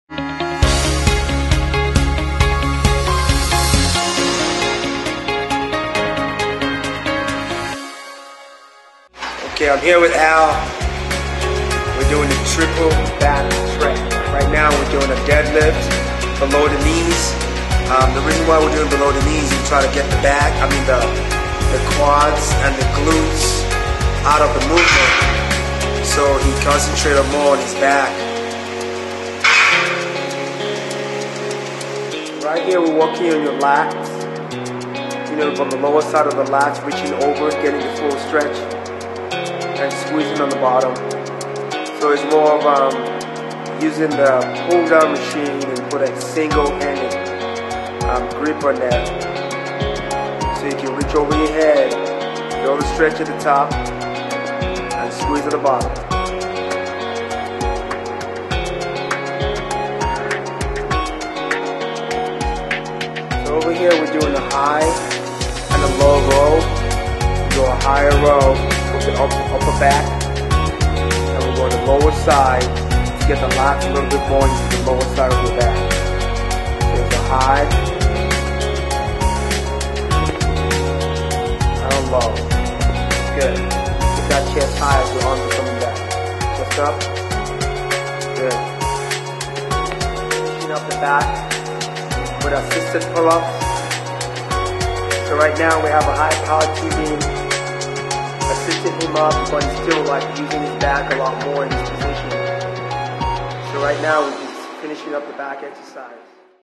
Giant Back Sets
In this video Moji shows a Giant Back Set (Superset) variation that can be used in the Miami Muscle Workout Schedule.

Or when you are ready go get the full program and learn to shape your body from the pro, join us at: